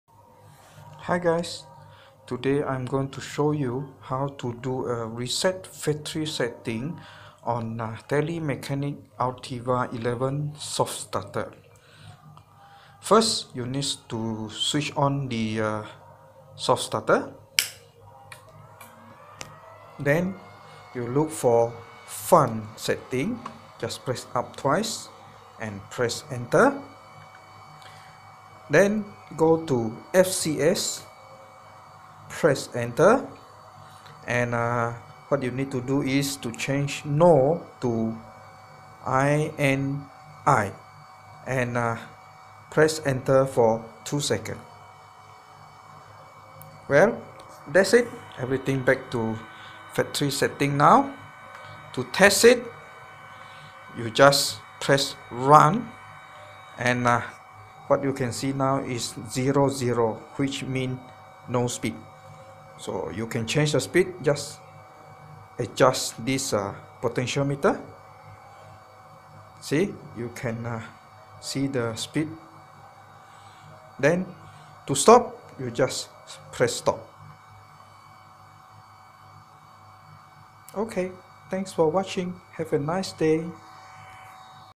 How to reset Telemecanique Altivar 11 back to factory defaults
1. FUn
2. FCS
3. Change nO to In l
4. Press enter key and hold for 2s.